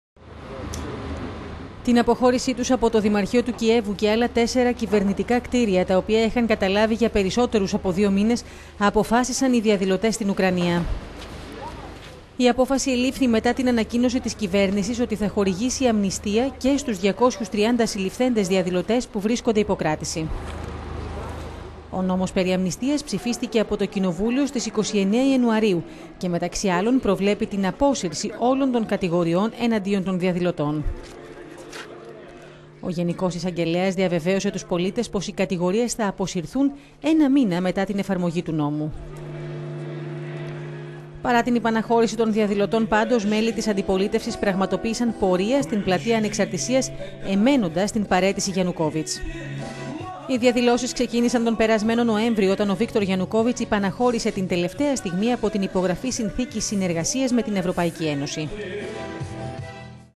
Ουκρανία: Βαίνει προς εκτόνωση η κρίση- Αποχώρησαν οι διαδηλωτές από το Δημαρχείο του Κιέβου
Την αποχώρηση τους από το Δημαρχείο του Κιέβου και άλλα τέσσερα κυβερνητικά κτίρια, τα οποία είχαν καταλάβει για περισσότερο από δύο μήνες, αποφάσισαν οι διαδηλωτές στην Ουκρανία.

Η απόφαση ελήφθη μετά την ανακοίνωση της κυβέρνησης ότι θα χορηγήσει αμνηστία και στους 230 συλληφθέντες διαδηλωτές που βρίσκονται υπό κράτηση.

Ο νόμος περί αμνηστίας ψηφίστηκε από το κοινοβούλιο στις 29 Ιανουαρίου και μεταξύ άλλων προβλέπει την απόσυρση όλων των κατηγοριών εναντίον των διαδηλωτών.

Ο γενικός εισαγ...